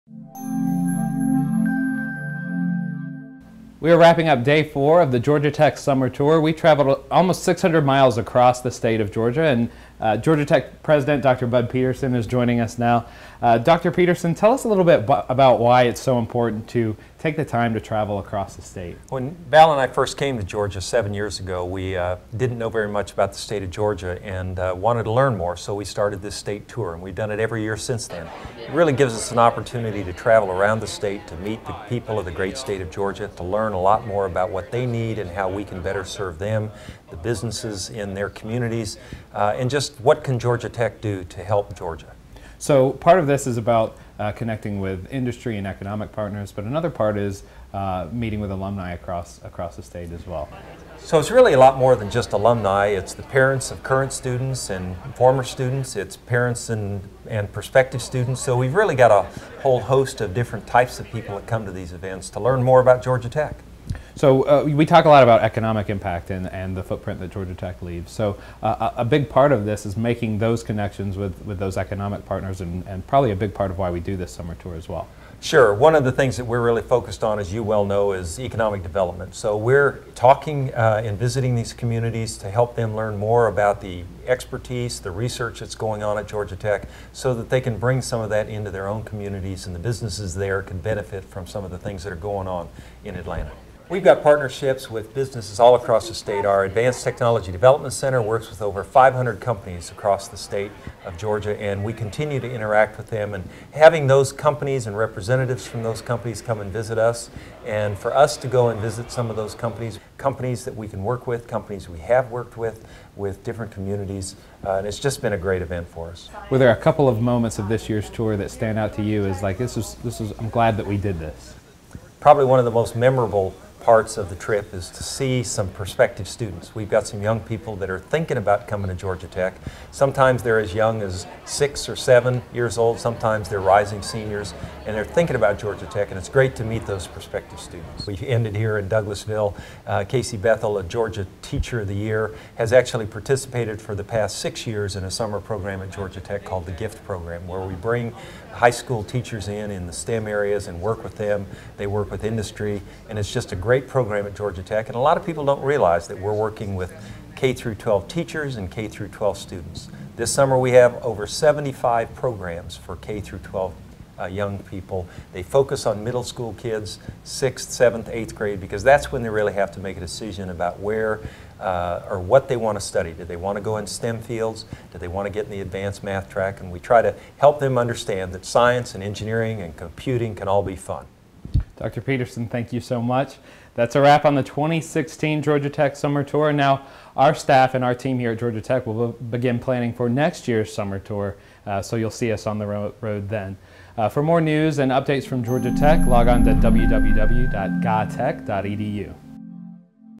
President G.P. "Bud" Peterson wraps up the 2016 Georgia Tour
The 2016 Georgia Tour was held June 20-23 in Macon and the greater Metro Atlanta area. President G.P. "Bud" Peterson provides a recap on the final day of the eighth annual tour.